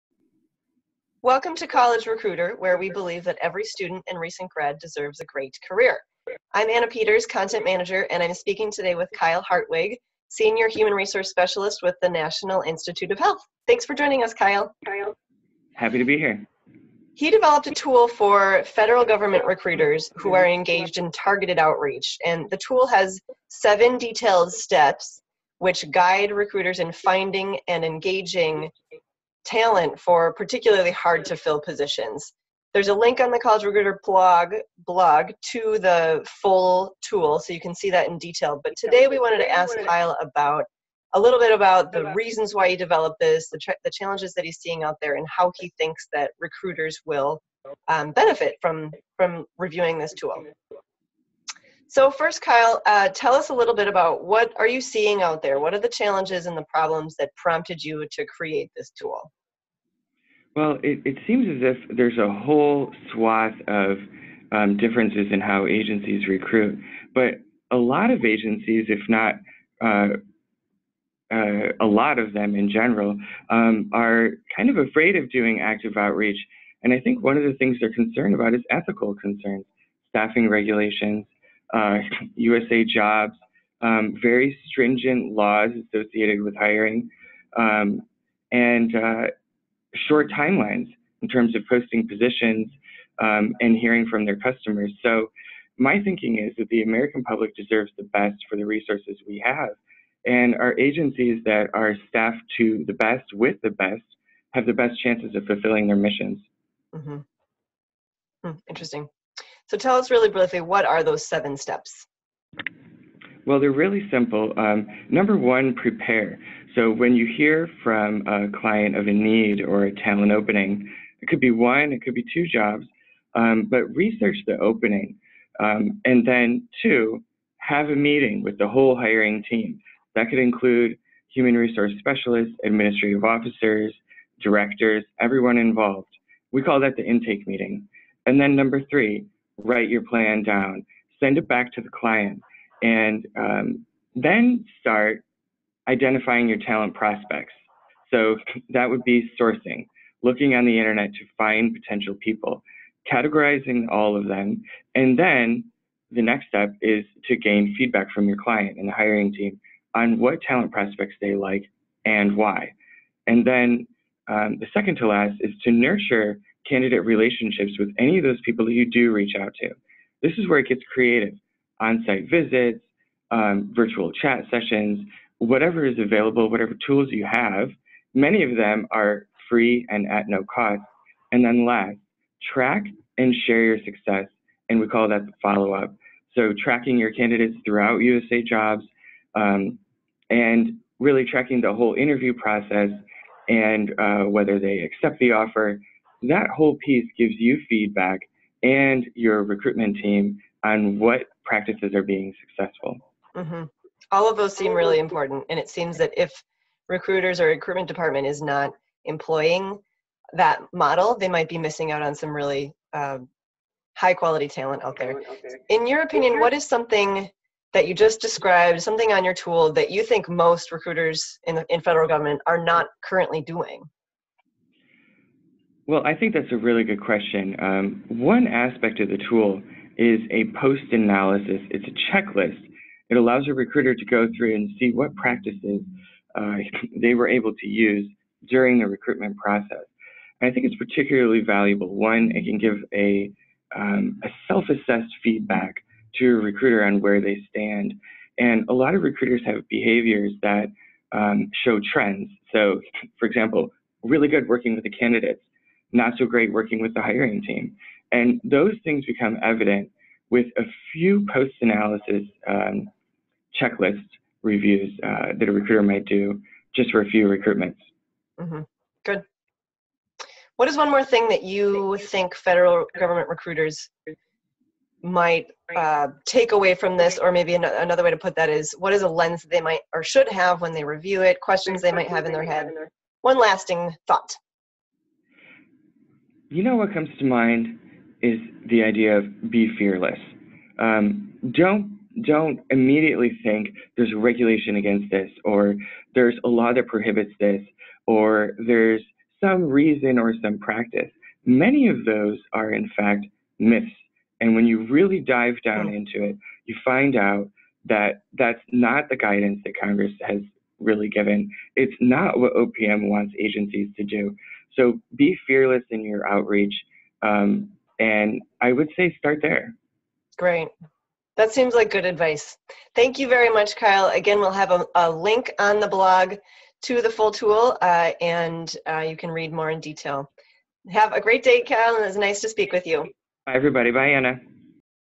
7 steps to successful federal government recruiting
To be a successful in federal government recruiting, you need deep knowledge of staffing systems and federal hiring practices and laws. However, you must also be willing to use innovative technologies and alternatives to posting on USAJOBS. College Recruiter spoke with Kyle Hartwig, ­­­­­­Senior Human Resource Specialist with the National Institute of Health (NIH). Hartwig developed a tool for federal government recruiters who are engaged in targeted outreach. The tool has 7 steps which guide recruiters in finding and engaging talent for hard-to-fill positions.

This tool attempts to address unique challenges in federal government recruiting.

According to Hartwig, a lot of agencies are afraid of doing active outreach. The reason is that they are concerned about is ethics. There are very stringent laws associated with hiring. Thus, HR specialists in agencies across government often shy away from taking real steps to find talent for unique roles. More often than not, many federal agencies don’t feel they have the freedom to recruit and find their own talent. With strict or even confusing federal staffing regulations, recruiters often opt for simply posting the opening on USAJOBS, or a few other places. After that, they just wait to see who applies. Hartwig says he built this tool because “we owe the American public our best efforts to keep our agencies fully staffed and running at capacity to fulfill their missions.”

In another vein, many federal HR specialists are unaware of the specific competencies necessary in each role they recruit for. Understanding those competencies would allow them to actively pursue ideal talent. Hartwig says that this is possible. “I’ve found proactive and specific talent sourcing and outreach to be a challenge in the Federal government but not impossible.” His tool allows HR specialists to use best practices while also following federal guidelines.

Don’t buy into the myths.
Congress certainly provides guidance for government recruiting and hiring practices, and we definitely need to follow all regulations. But there are myths out there that perpetuate assumptions about recruitment and hiring practices. Hartwig says, “Don’t immediately think there’s a regulation against this, or there’s a law that prohibits this,” or there is a reason not to employ certain recruitment practices. Many times, Hartwig says, if you investigate further, you’ll find these are myths. Don’t be afraid to expand your outreach to beef up your candidate list. Hartwig says he is often confronted with the assumption that outreach is easy. Wrong. Outreach—the kind that results in high quality hires—takes a lot of work. “Working with an entire hiring team, and finding the exact target skills of the desired candidate is not easy.” The 7-step tool, however, breaks it down into a methodical process.

The seven steps in the tool are not ground breaking. They are quite simple:
Prepare. This includes researching the opening and understanding the competencies needed to perform in the role.
Meet with the hiring team. This includes HR, hiring managers or any subject matter experts.
Write your plan. If you are only posting on USAJOBS, you are missing out on a potentially wide pool of quality candidates.
Source. Identify talent prospects. Look on the Internet to find potential candidates and categorize them.
Ask for feedback. Make sure the hiring team explains their reasons for their feedback as well.
Nurture candidate relationships. Get creative to engage candidates. You can invite them to an on-site visit, chat with them virtually, etc. There are many no-cost ways to engage candidates.
Follow up. It’s essential to track your progress and share what is successful. This record will give you and the whole team feedback to improve in the future.